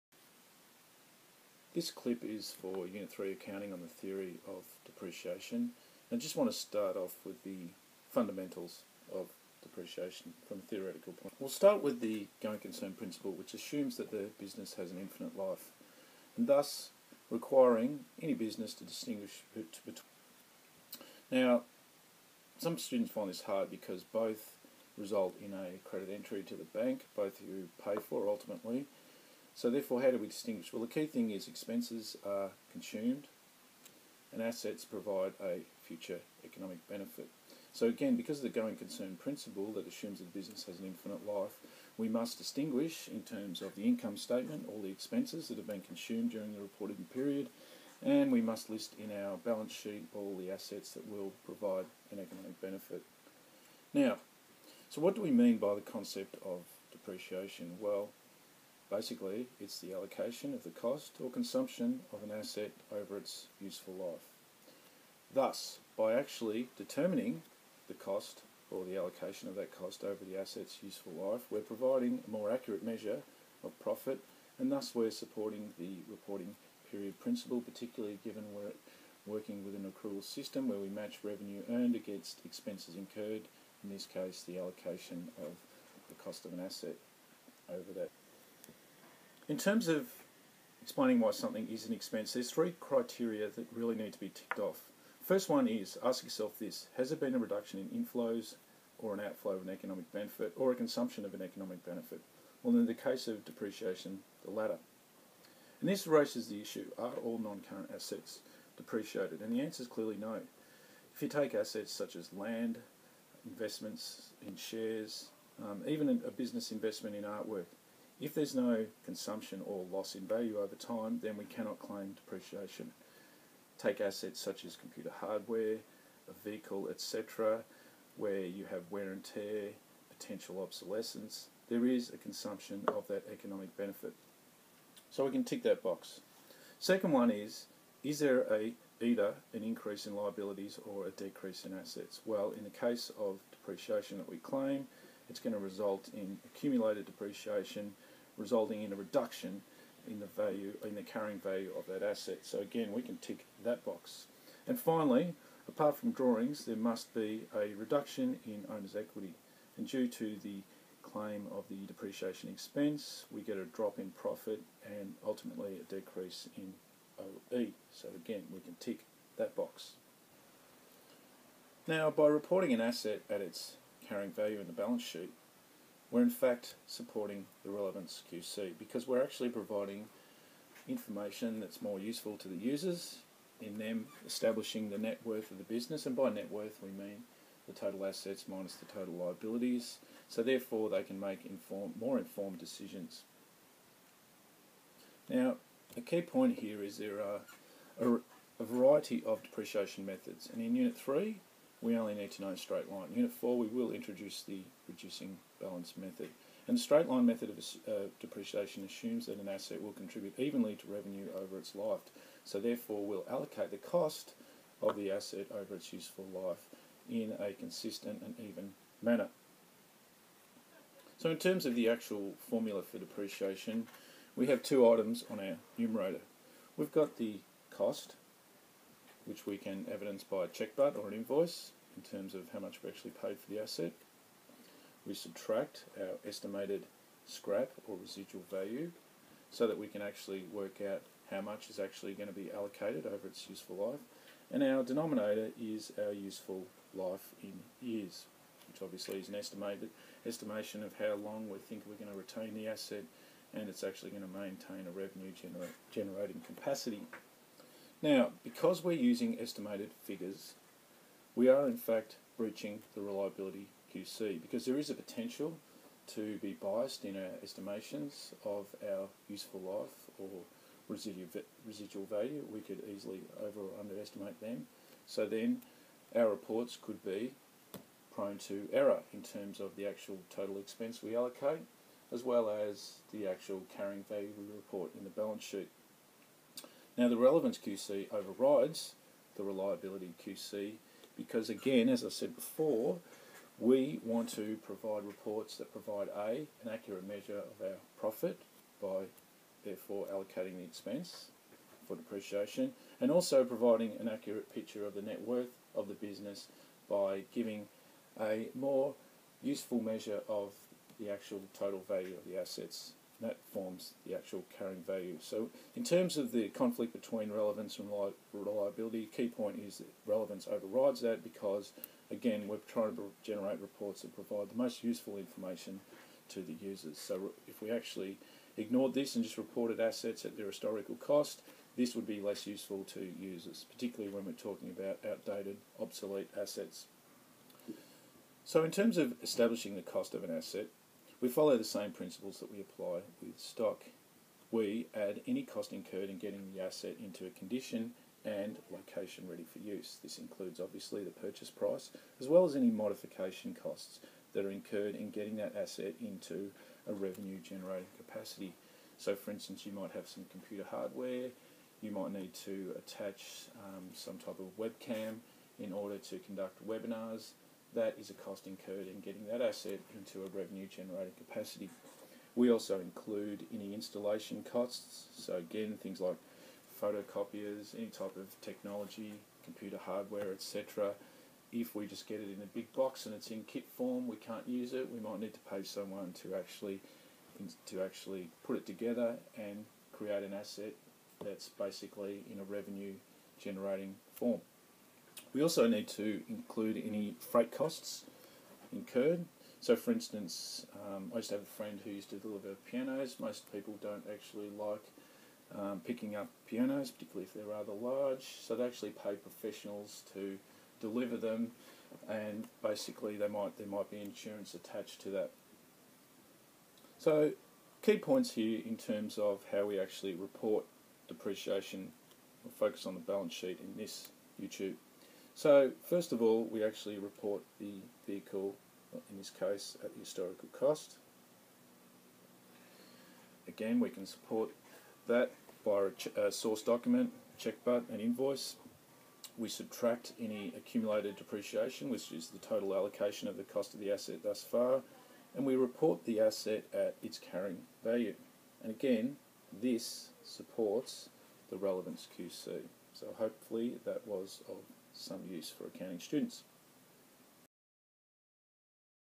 Depreciation theory - VCE Accounting
This clip explains the meaning of depreciation, why we claim depreciation on NCA's - with a link to the relevant principles & QC's